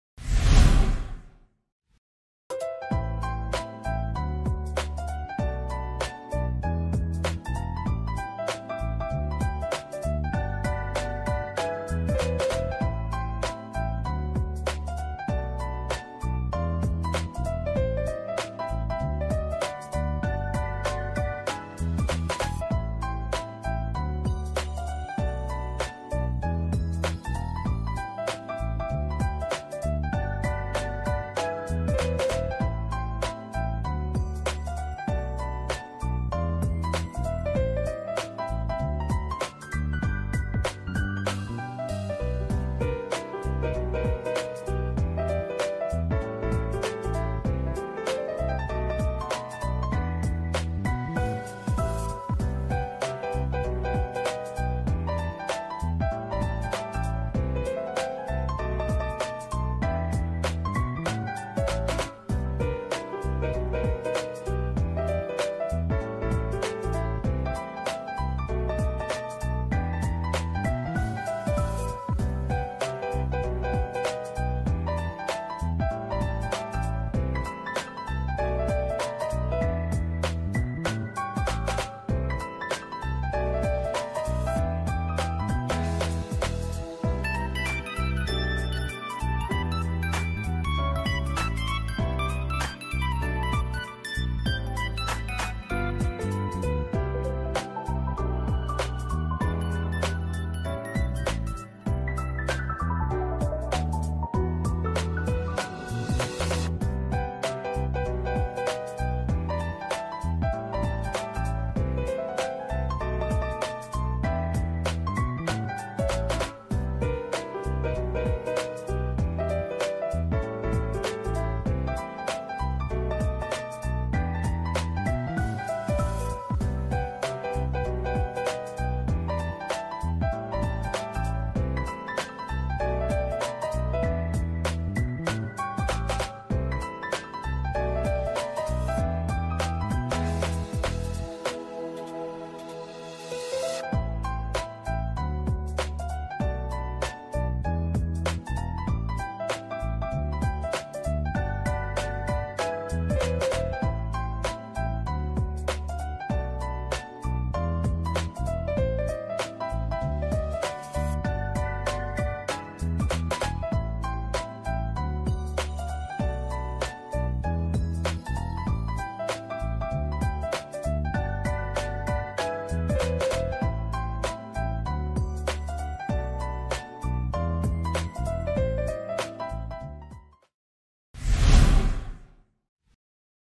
Operational video- Vacuum Centrifugal Concentrator, VCC-2000
Speed:2000r/min
Control Range:RT.~ +60°C or without heating
Final VacuumDegree:0.1mbar
Rotor Capacity:6×250mL/6×50mL, 12×10mL/62×5mL, 2×48well/2×96well